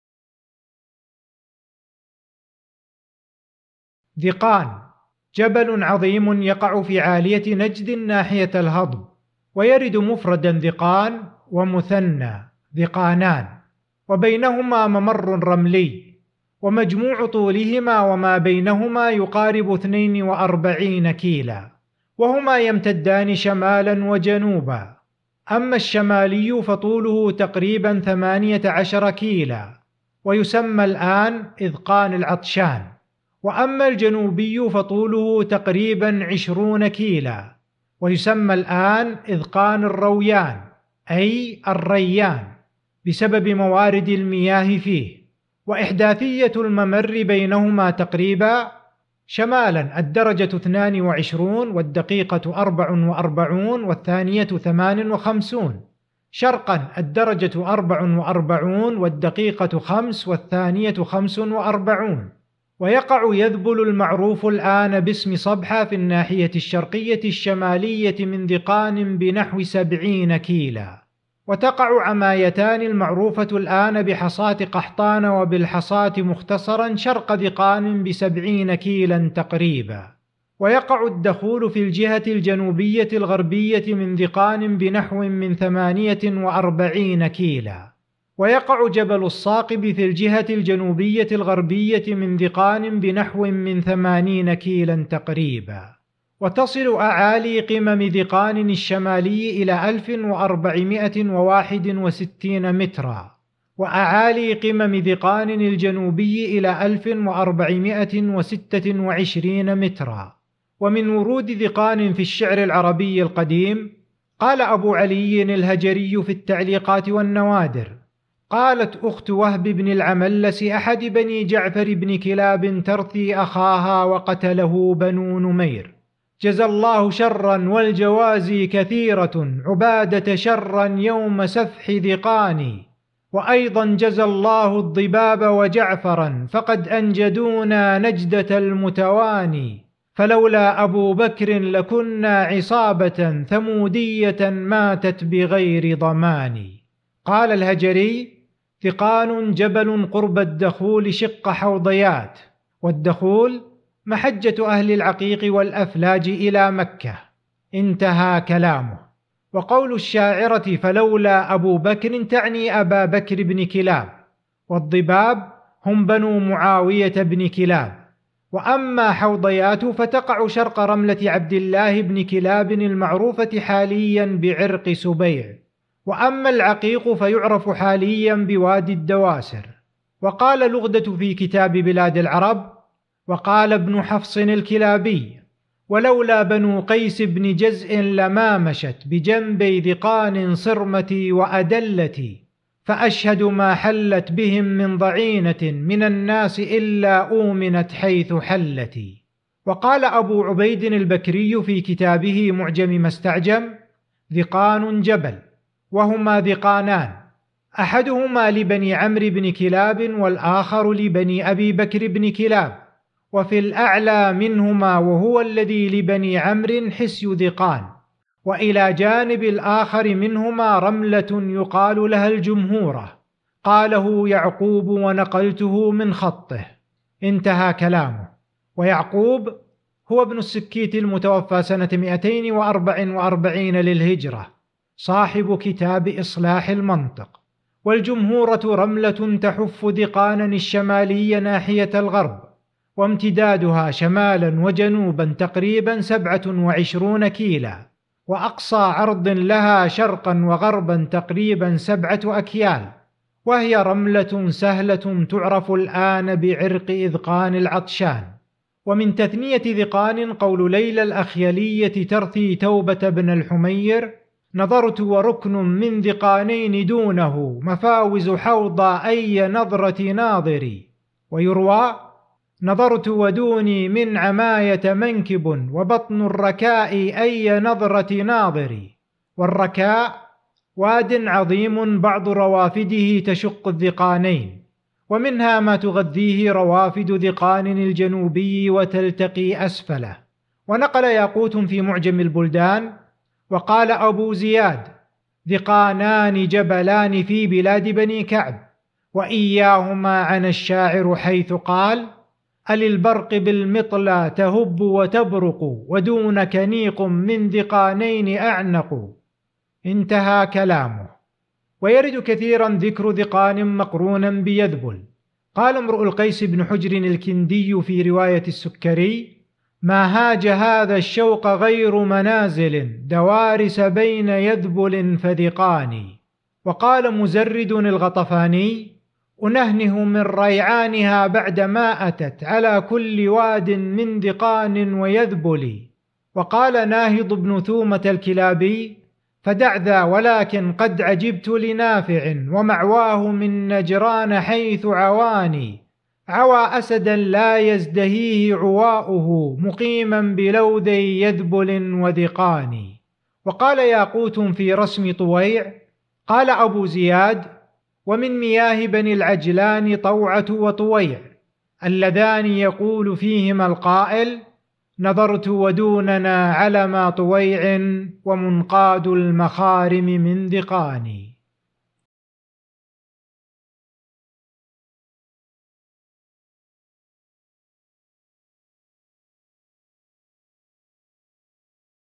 (٦) جبل ذقان (ذقانين)، التعريف المختصر بالمواضع في في الشعر القديم
التعريف المختصر بـ
(٦) جبل ذقان (ذقانين)

إعداد

جبل عظيم يقع في عالية نجد ناحية الهضب، ويرد مفردًا (ذقان) ومثنى (ذقانان)، وبينهما ممر رملي، ومجموع طولهما وما بينهما يقارب اثنين وأربعين كيلا، وهما يمتدان شمالا وجنوبًا، أما الشمالي فطوله تقريبًا 18كيلا ويسمى الآن إذقان العطشان، وأما الجنوبي فطوله تقريبًا 20 كيلا ويسمى إذقان الرويان (الريان) بسبب موراد المياه فيه.

وإحداثية الممر بينها تقريبًا:

شمالا الدرجة اثنان وعشرون، والدقيقة أربع وأربعون والثانية ثمان وخمسون

شرقًا الدرجة أربع وأربعون، والدقيقة خمس، والثانية خمس وأربعون

N22 44 58 E44 05 45

شكرًا لصديق الرحلة محمد بن سعد الخالدي